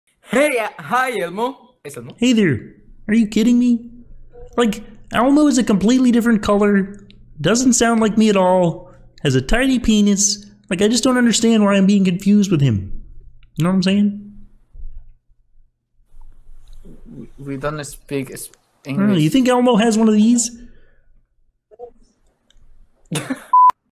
Kermit Omegle Hates Elmo
Kermit hates being called Elmo...
Omegle